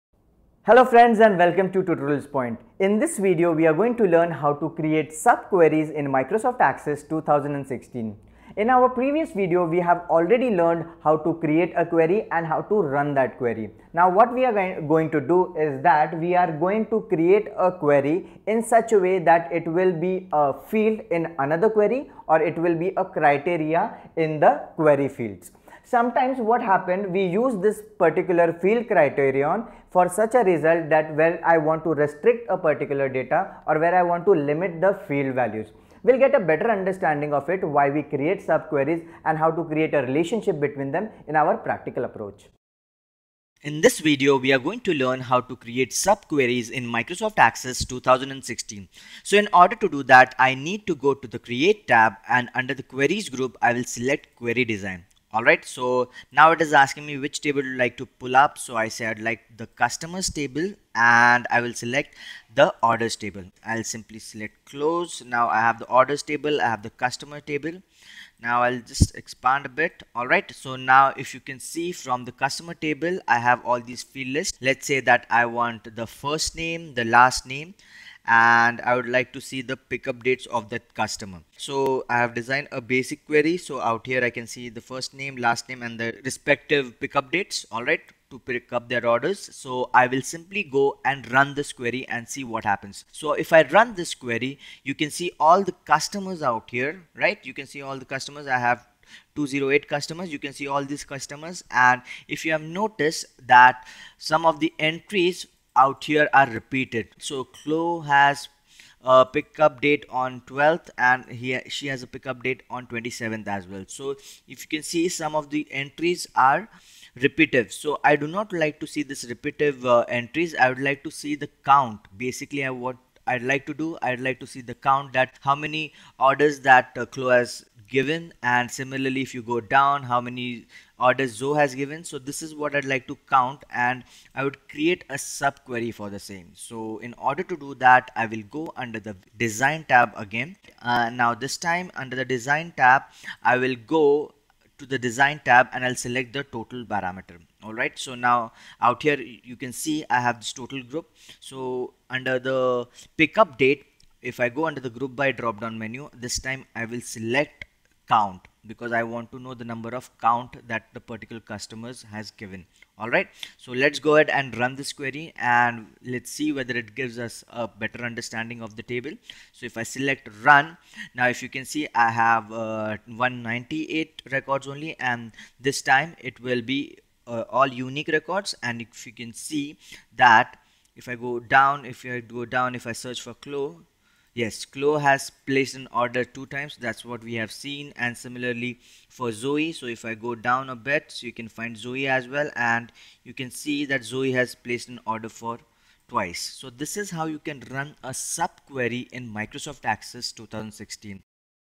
MS Access 2016 - Create Subqueries
Lecture By: Mr. Akshay Magre, Tutorials Point India Private Limited.